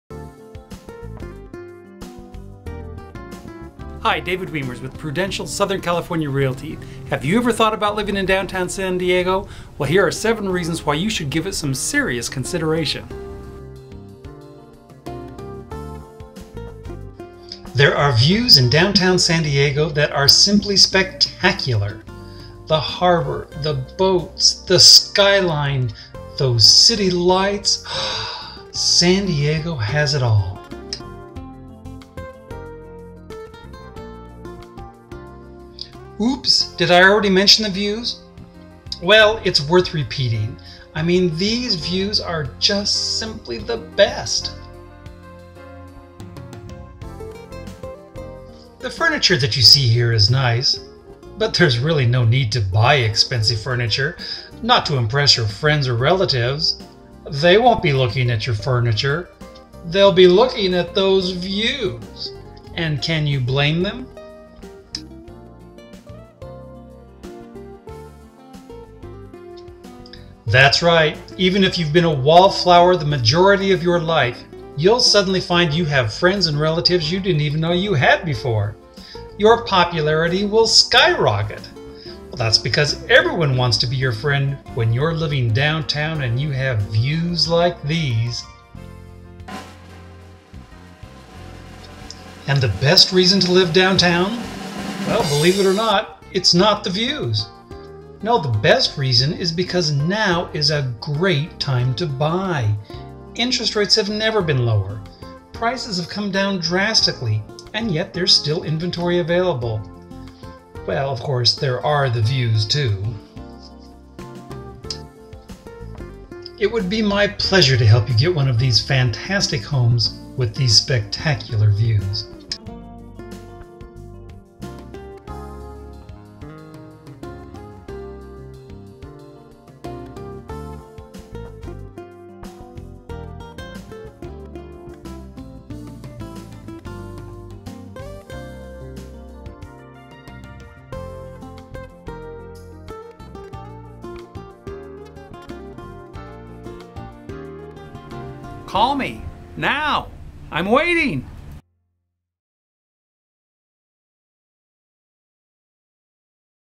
7 Reasons to Live Downtown San Diego
Living downtown San Diego isn't for everyone...only for those who want to live a vibrant, exciting, active and fun life with easy access to shopping, dining, and transportation. Why live anywhere else?